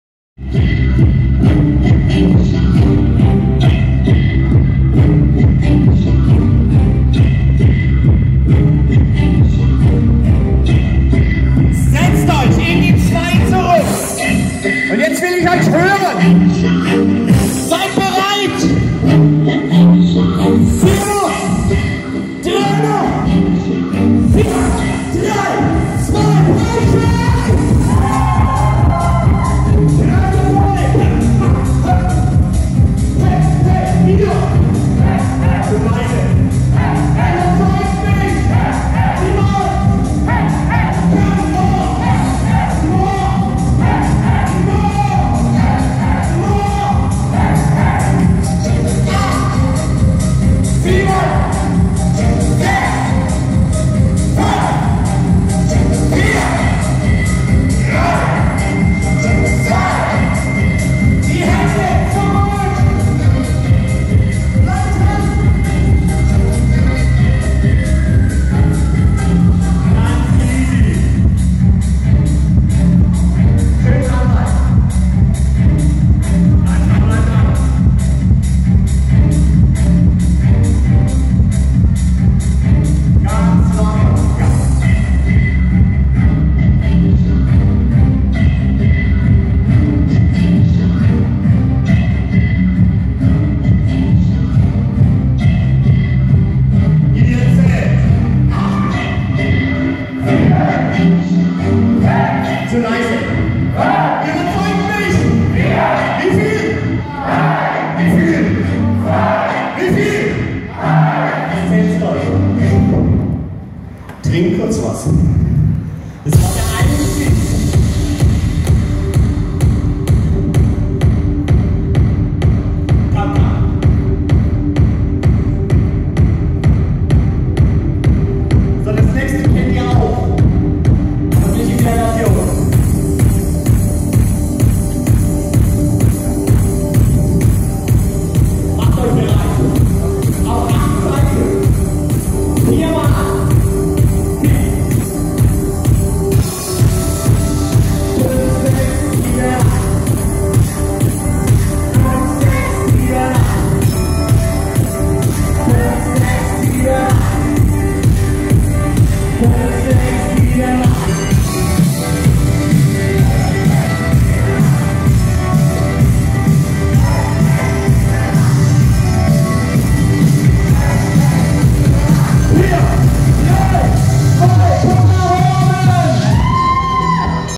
6h Breitscheid Indoorcycling Marathon 2015
What a cool 6h INDOORCYCLING Marathon Location in width Scheid / GERMANY has a lot of fun for all of us it was for a good cause genuine respect

there was much joy and balance to feel :-)

The next Indoor Cycling Marathon calls next week in Fürth goes over 3 days I am looking forward

and then comes Schiedam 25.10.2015
Netherlands Spinning ( R ) Marathon

Was für eine coole 6h INDOORCYCLING Marathon Location in Breitenscheid / GERMANY hat sehr viel Spaß gemacht uns allen es war auch für ein Guten zweck echt Respekt
es war sehr viel freude und Balance zu Spüren :-)

Der nächste Indoorcycling Marathon ruft schon nächste Woche in Fürth geht über 3 Tage freue mich schon
und dann kommt Schiedam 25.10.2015 Niederlande Spinning(R) Marathon